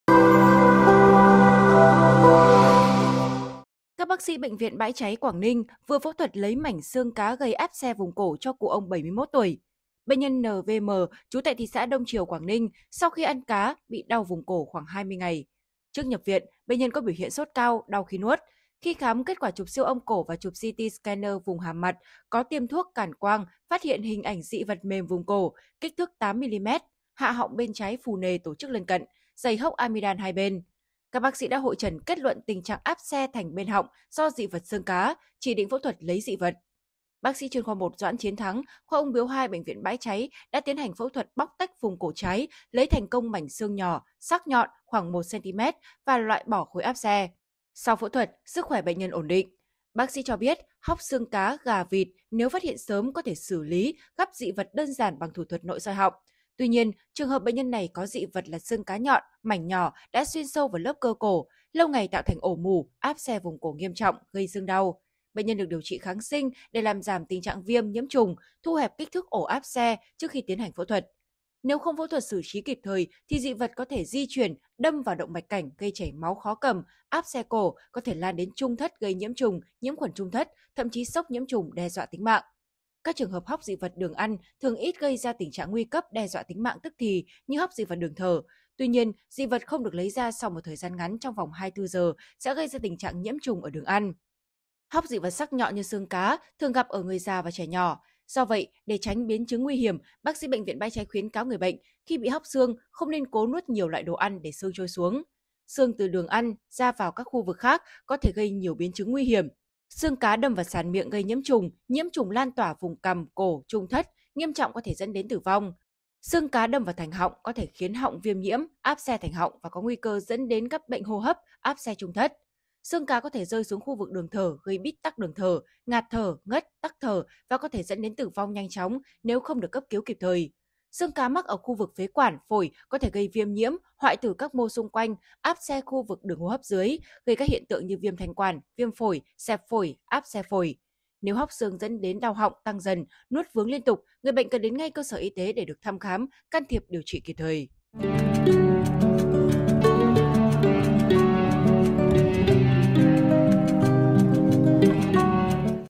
Cụ Ông Bị Áp Xe Vùng Cổ Sau Khi Nuốt Mảnh Xương Cá | SKĐS
SKĐS |  Bác sĩ Bệnh viện Bãi Cháy đã tiến hành phẫu thuật bóc tách vùng cổ trái, lấy thành công mảnh xương nhỏ mảnh, sắc nhọn khoảng 1cm và loại bỏ khối áp xe cho cụ ông 71 tuổi.